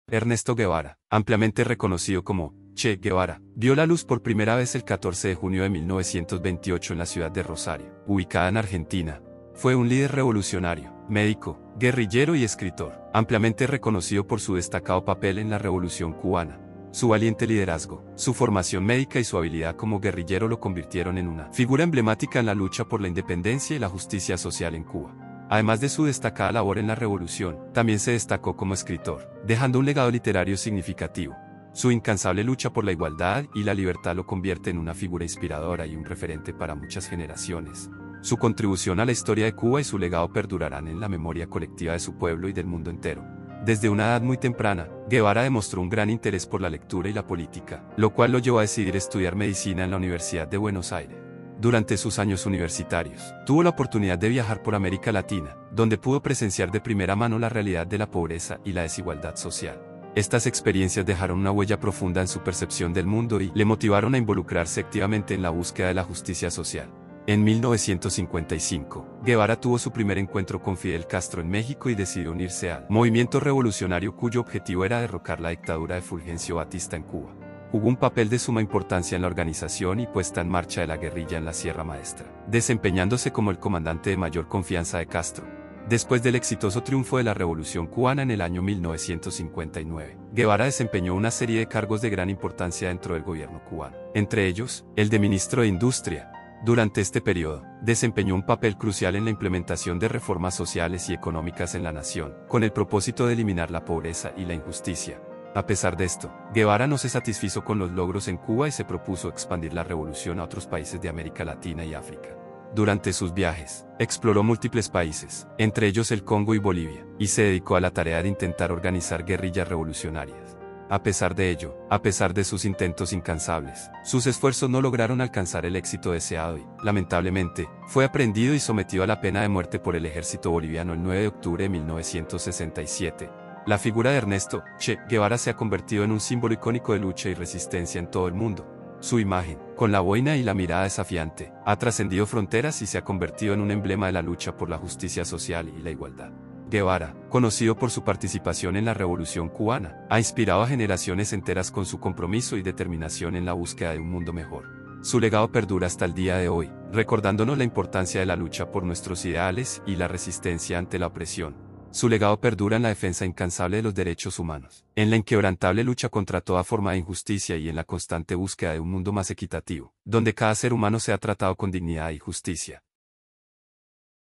Ernesto 'Che' Guevara: La Vida de un Revolucionario que Cambió la Historia
Sumérgete en la asombrosa vida de Ernesto 'Che' Guevara, un icono revolucionario cuyo legado perdura en la historia. Explora sus hazañas, ideales y su influencia en la lucha por la justicia social. ¡Acompáñanos en este viaje épico!